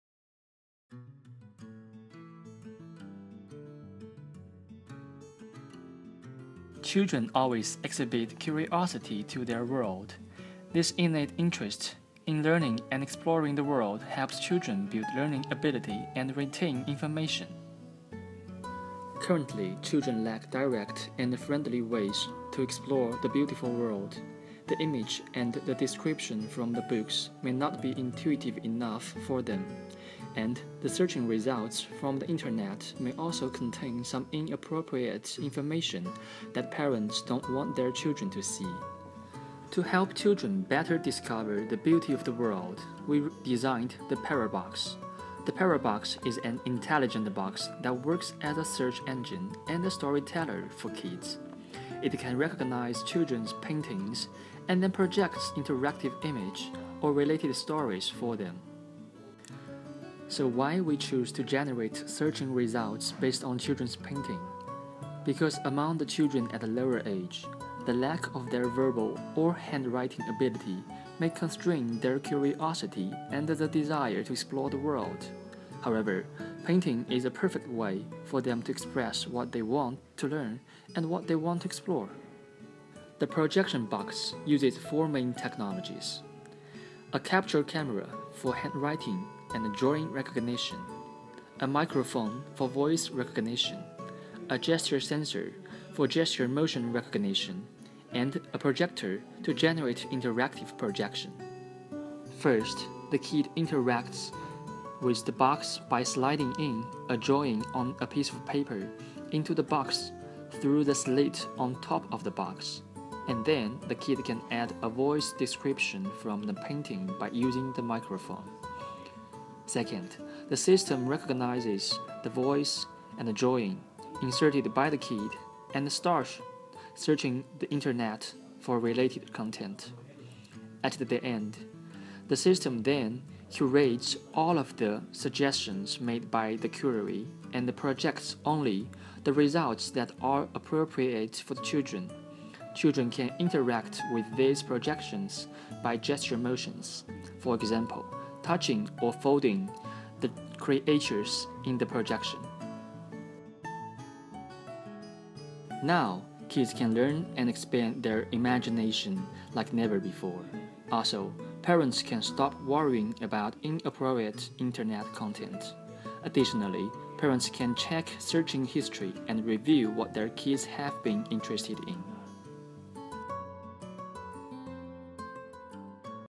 Pora - Convergence Innovation Competition
Pora is a simple box that works as a search engine and a story teller for kids.